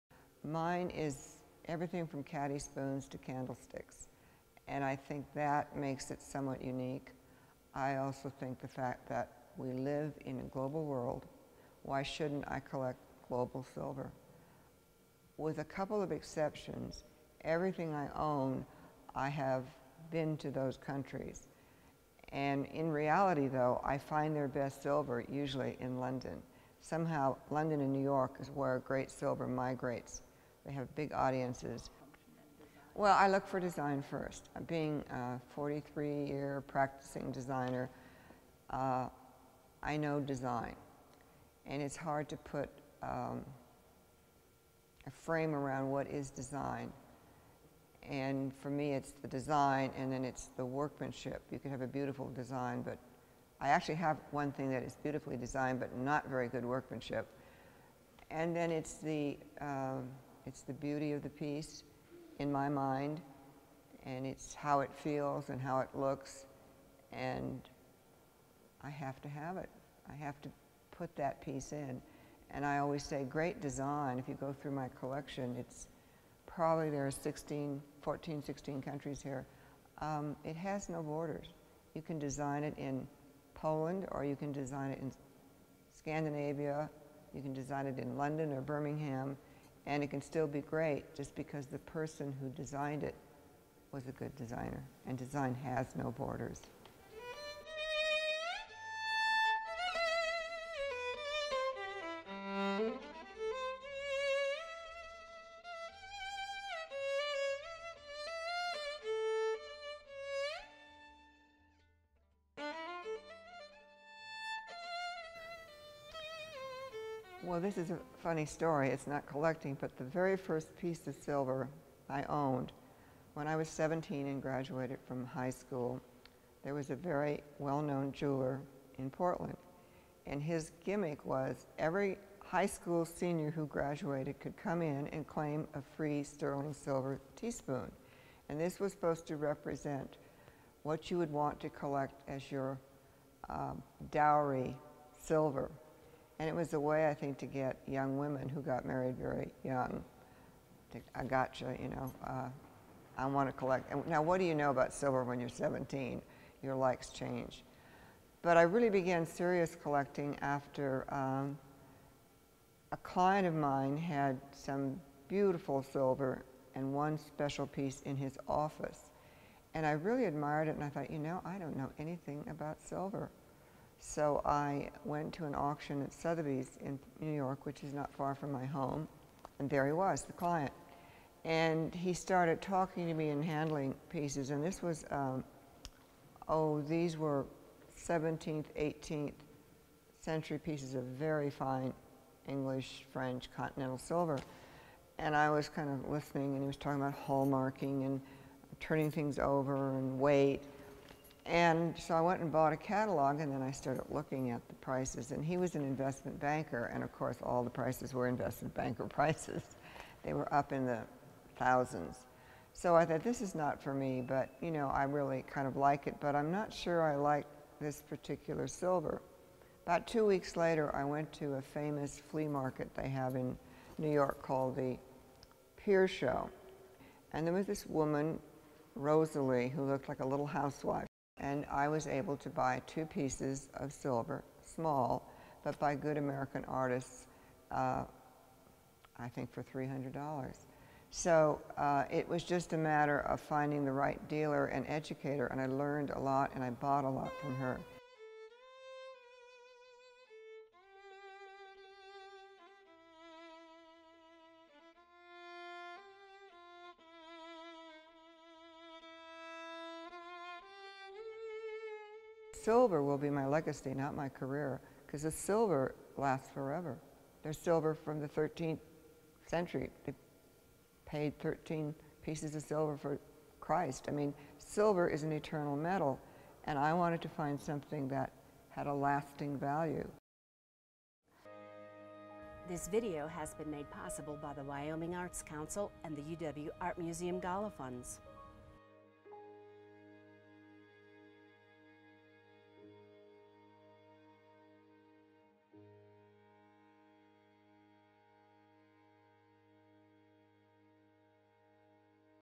Silver and Metalwork of the 20th Century: The Margo Grant Walsh Collection
Margo Grant Walsh began collecting silver on a whim, but she soon embraced the design of silver and metalwork objects with vigor. Not being a scholar, she became a self-educated collector. Starting modestly and relying on museums, exhibitions, books, antique shows, flea markets and dealers, she now has a world class collection of some of the finest silver from both the most famous and some of the most obscure artisans of the 20th century. Walsh has a global metalwork collection from 21 countries. As she explains it, we now truly, "live in a global world," and it is fitting to celebrate the great objects, all of which are functional, that were fashioned during the 20th century.